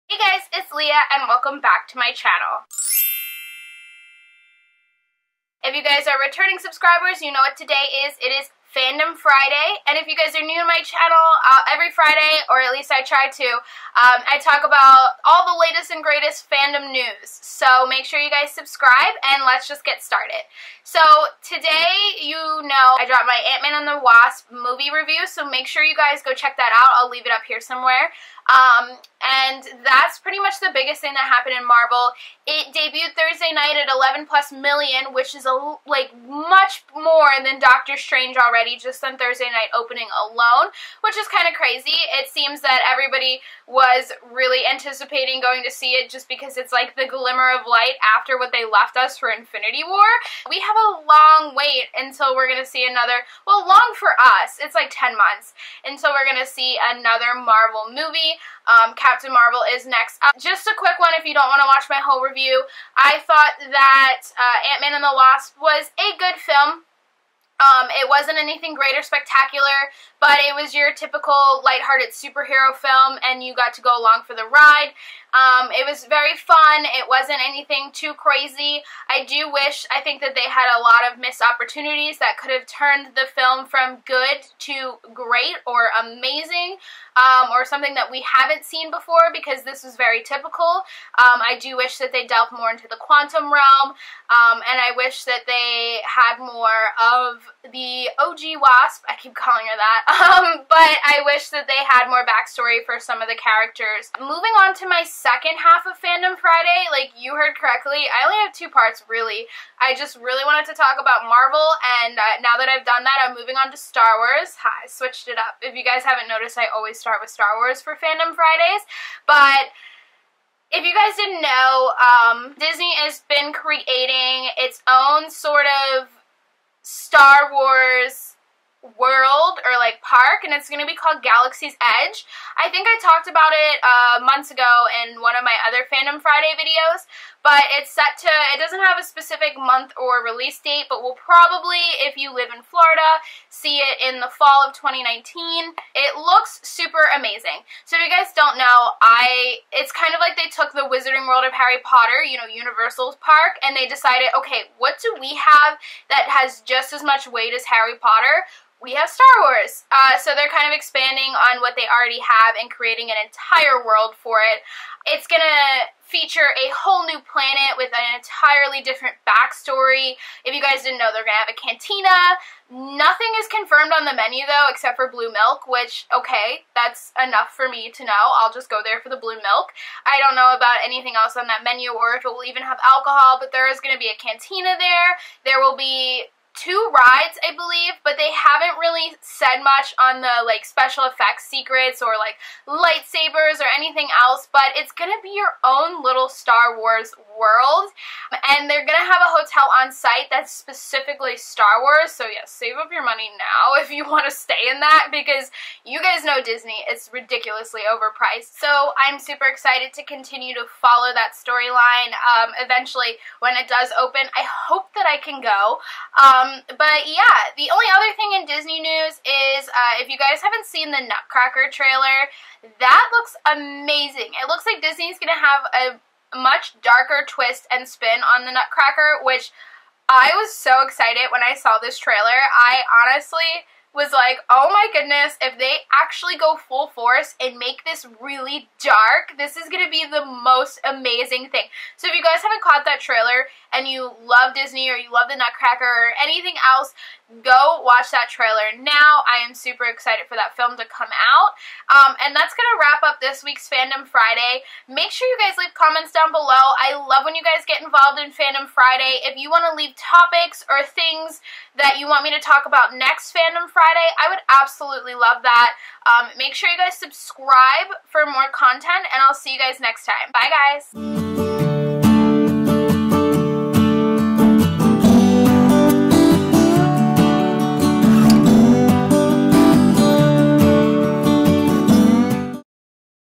Fandom Friday | Episode 12
Hey guys its Leah! Welcome back to another episode of Fandom Friday! I hope you all enjoy and join in my discussion of the latest Marvel and Star Wars news! I would love for you guys to start leaving comments down below of things you want me to talk about in Fandom Friday videos!